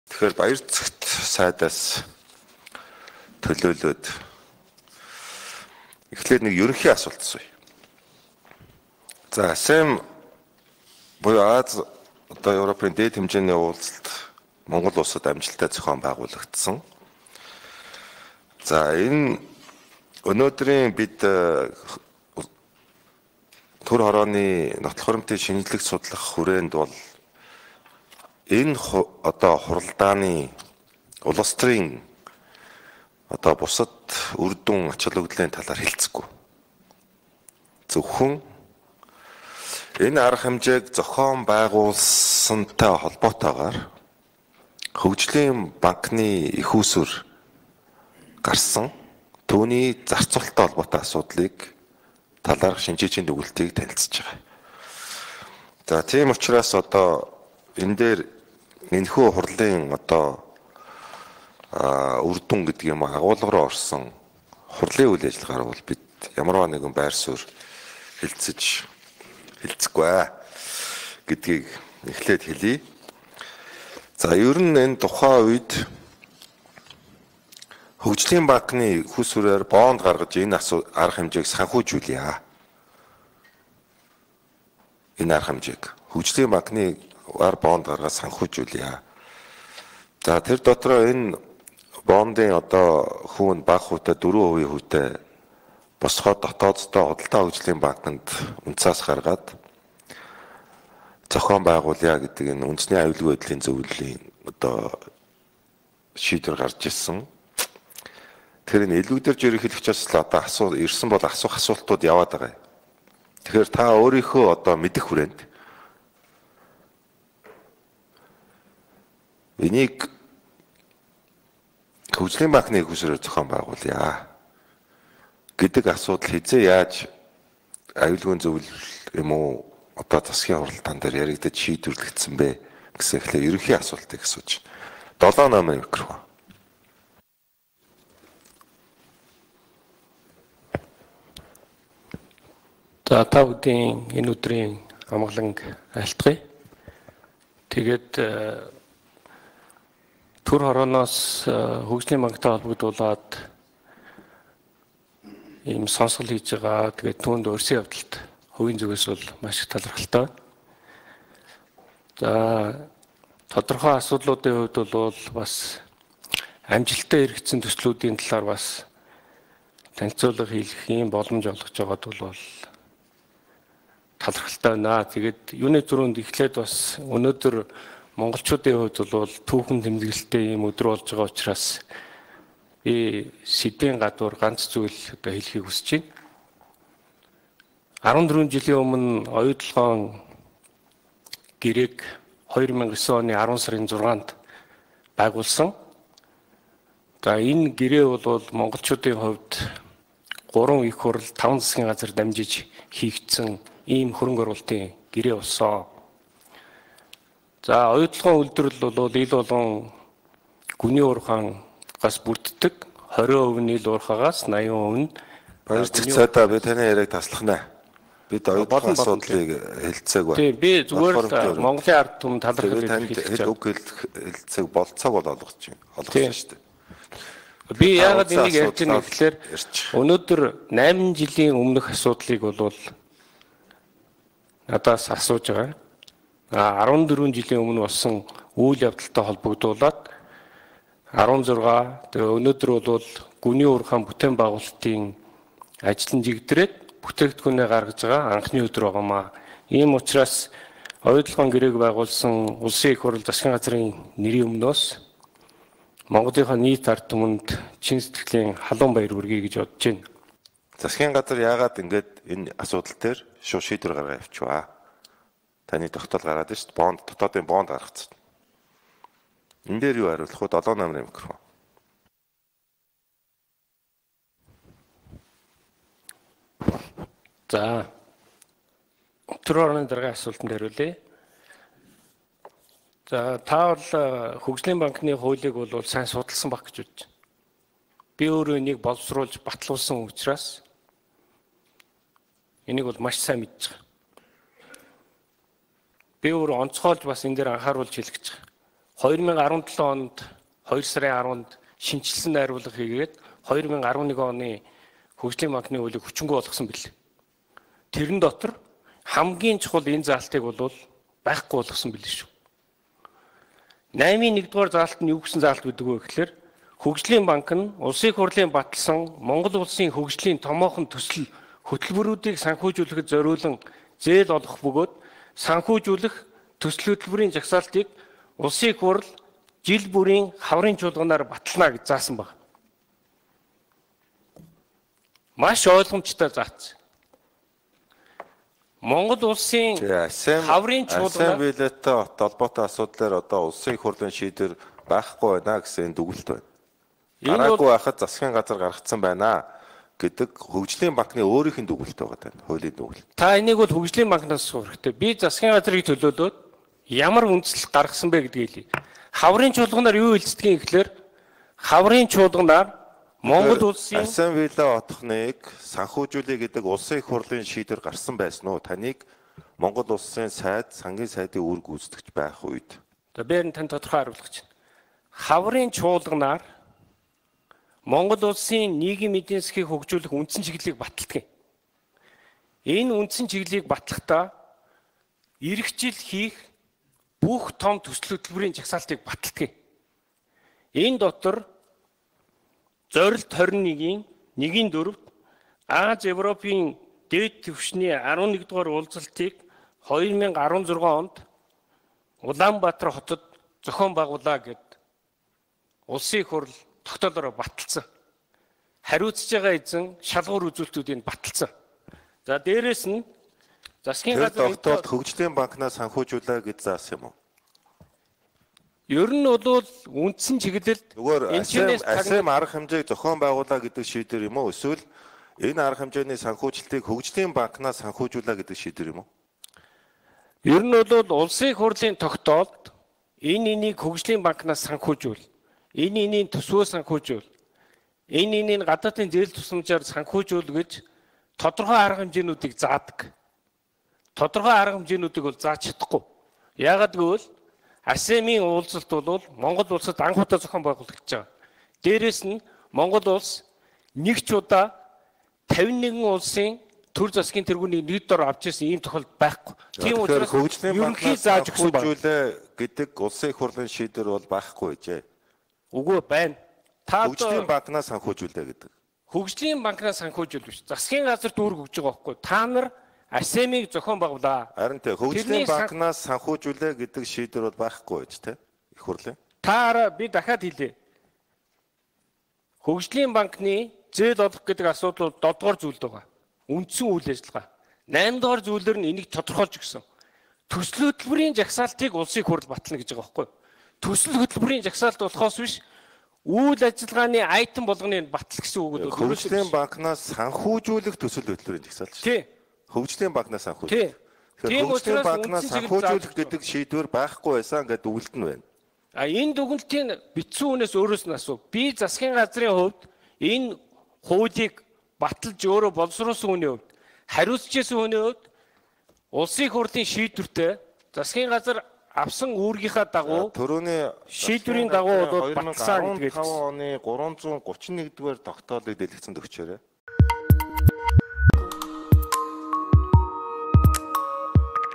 Б.Энхбаяр VS С.Баярцогт
Б.Энхбаяр: АСЕМ виллаг “Хөгжлийн банкнаас” санхүүжүүлнэ гэж заасан юм уу?
С.Баярцогт: Ерөнхийд нь зааж өгсөн юм байхгүй. Гэхдээ УИХ-аас ЗГ-т хариуцуулсан ажил.